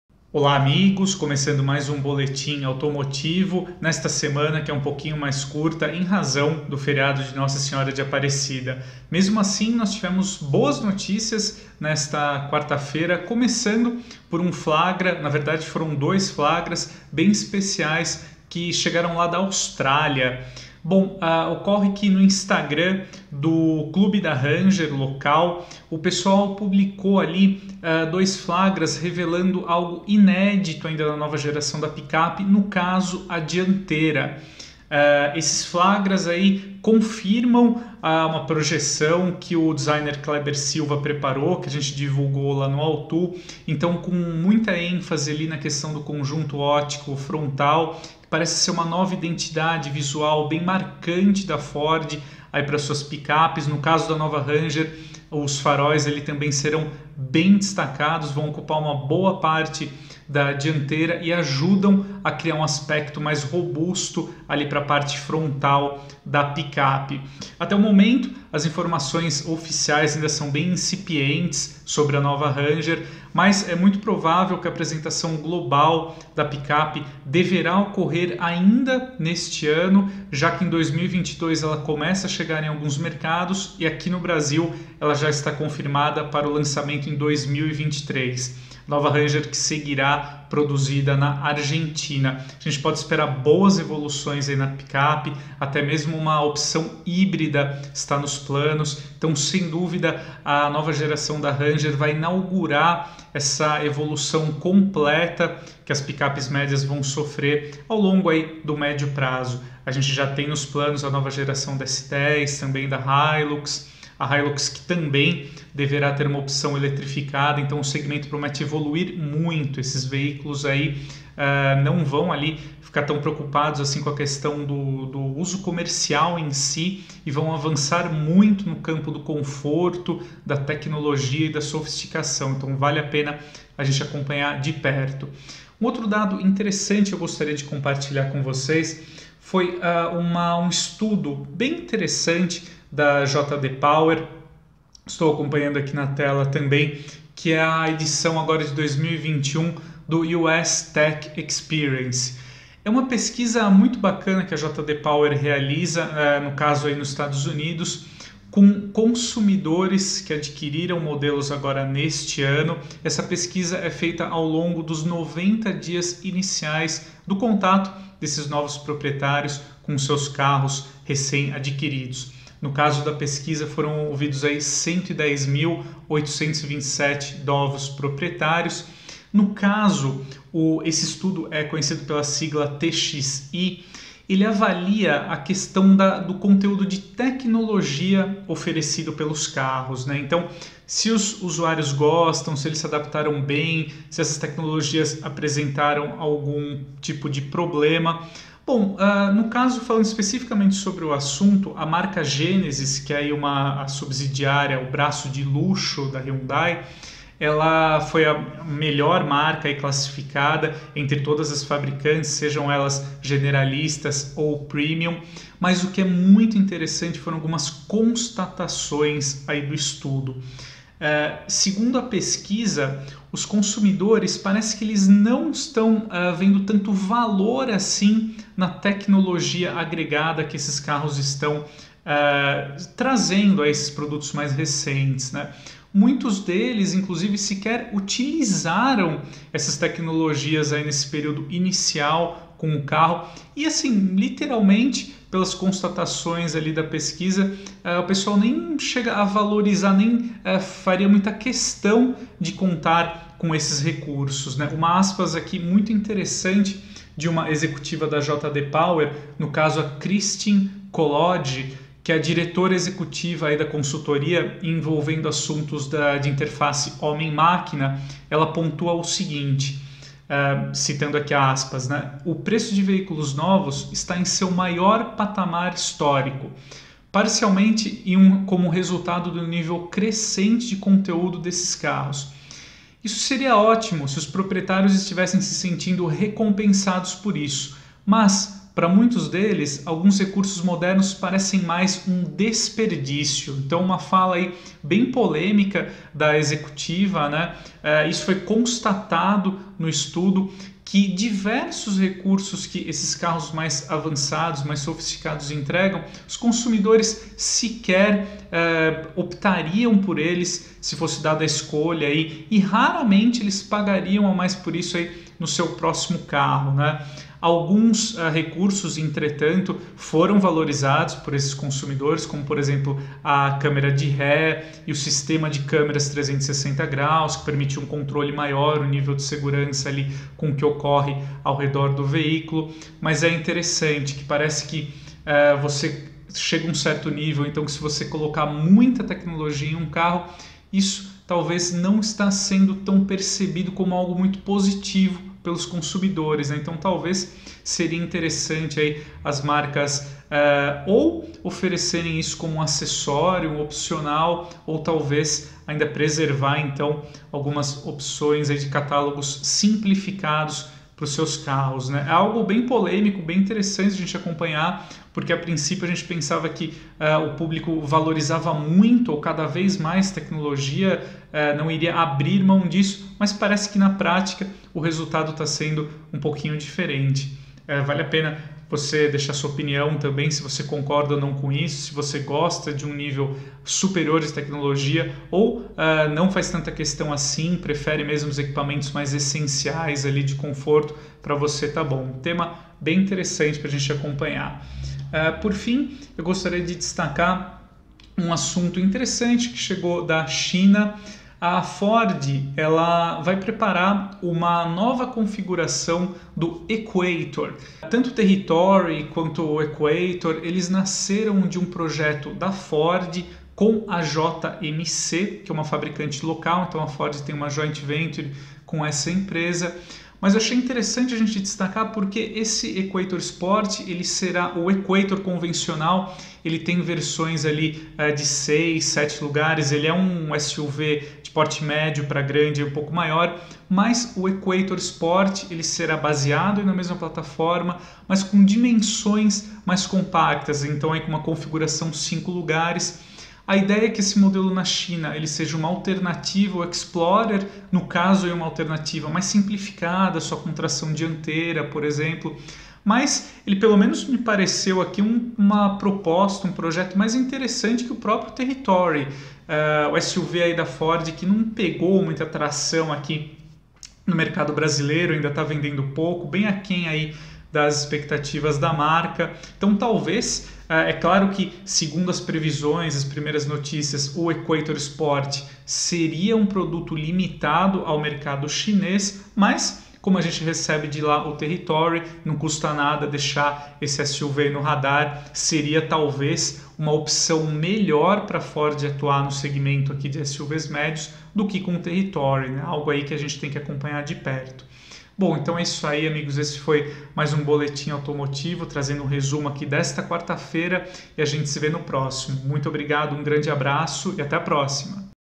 Boletim Automotivo #11 - Flagra da nova Ford Ranger! | Muita tecnologia nos carros é um problema?
Resumo de notícias do setor automotivo desta quarta-feira, 13/10/2021